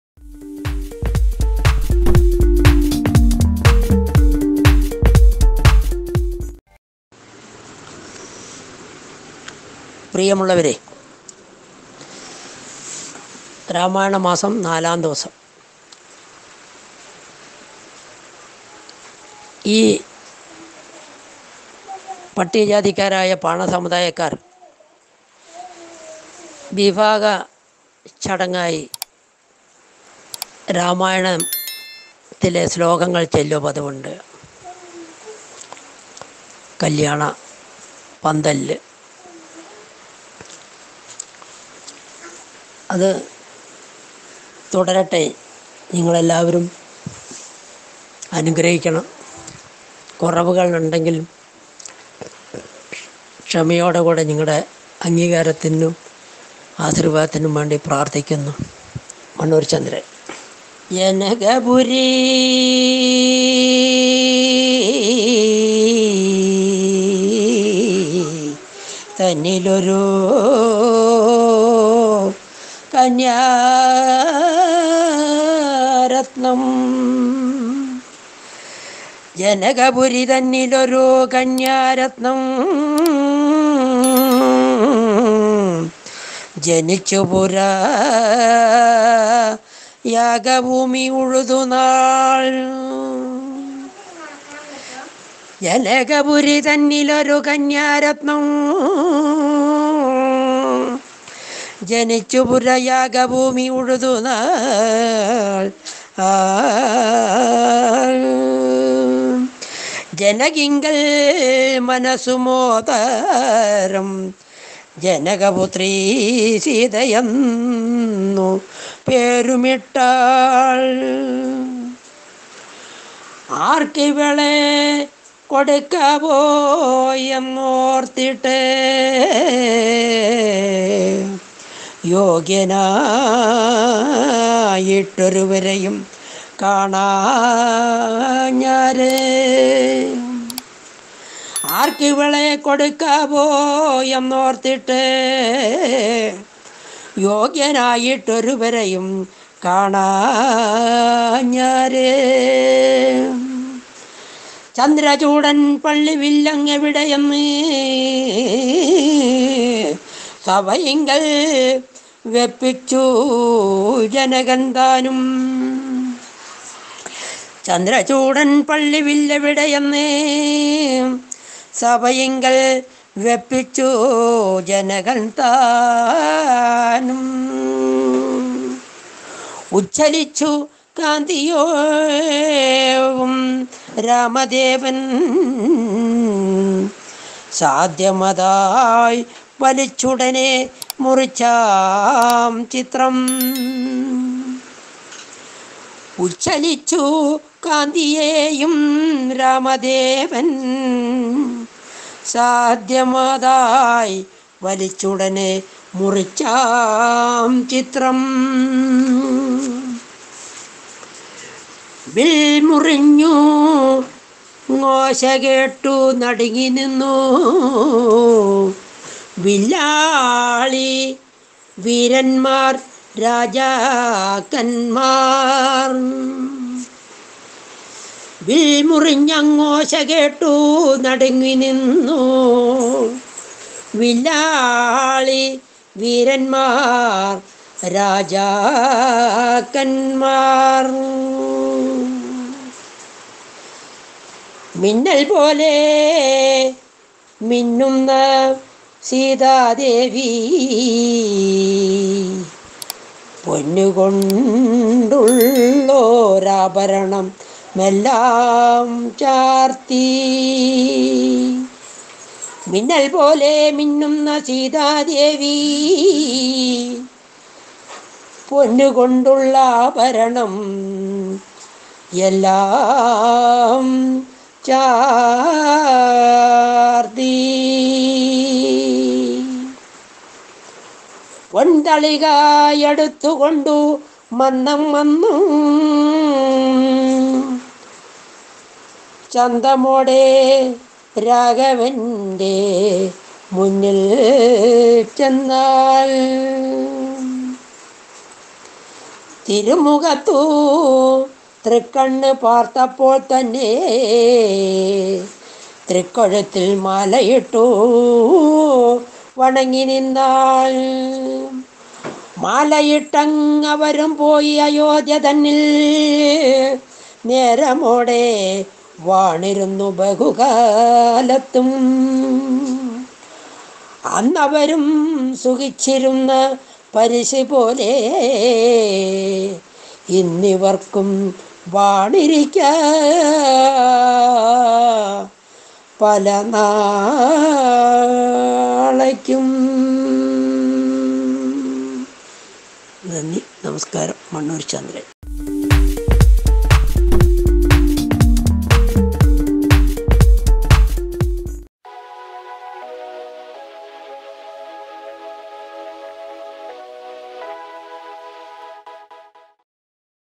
പൊറാട്ട് രാമായണം | EPISODE 04 | PORAATT RAMAYANAM | മണ്ണൂർ ചന്ദ്രൻ | VIEWMASTER
തീണ്ടാപാടകലെ നിന്ന്,ചേറും ചോറും തമ്മിൽ കൂട്ടിക്കുഴച്ചുണ്ടിരുന്ന ചിലർ ആടുകയും പാടുകയും ചെയ്തിരുന്നു. അവരുടെ നാവിലും രാമായണം വഴങ്ങിയിരുന്നു. പൊറാട്ടു കളി രംഗത്ത് ആശാനായി,അവാർഡ് ജേതാവായി...പൊറാട്ട് ജീവവായുവായി ജീവിക്കുന്ന ശ്രീ.മണ്ണൂർ ചന്ദ്രേട്ടൻ പൊറാട്ടുകളിയിൽ ഇടയ്ക്കും തലയ്ക്കുമായി വരുന്ന രാമായണ കഥയുമായി ബന്ധപ്പെടുത്തി വരുന്ന കാവ്യ പൊന്മൊഴികളെ പകർന്നു തരുന്നു...ഭാഷാ വിദ്യാർത്ഥികൾക്ക് ഗുണകരമാവും